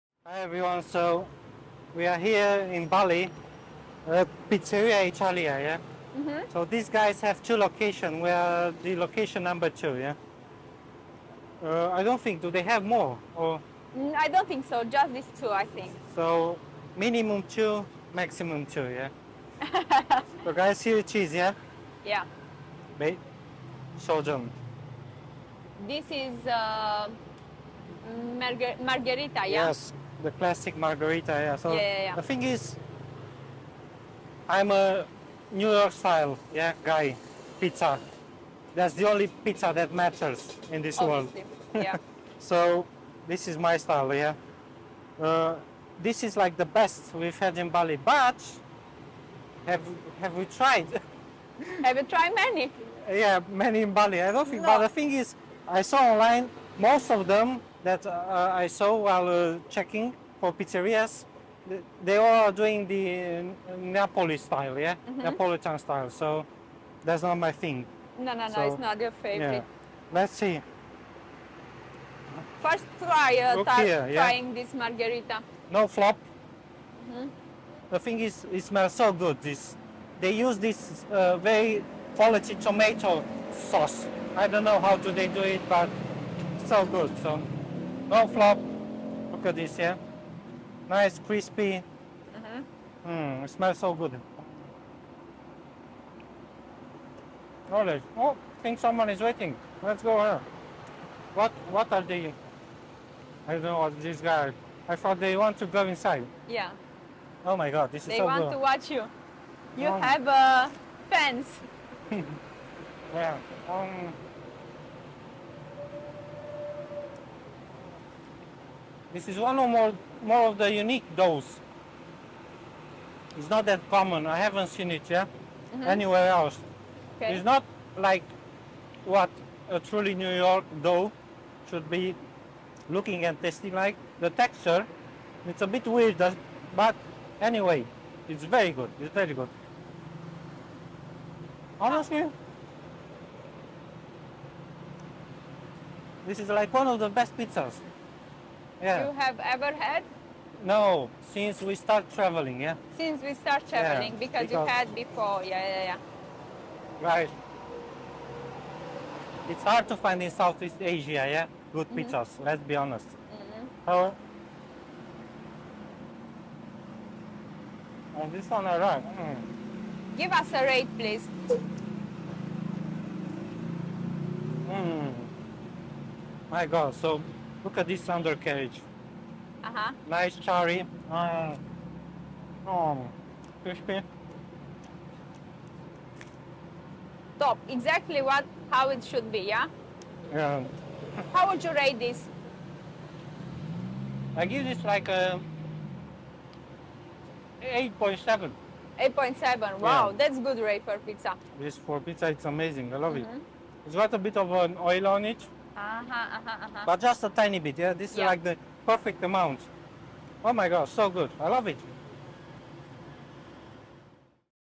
PIZZA around the WORLD: Pizzeria Italia, Bali, Indonesia
In this video we present you PIZZA around the WORLD: Pizzeria Italia, Bali, Indonesia.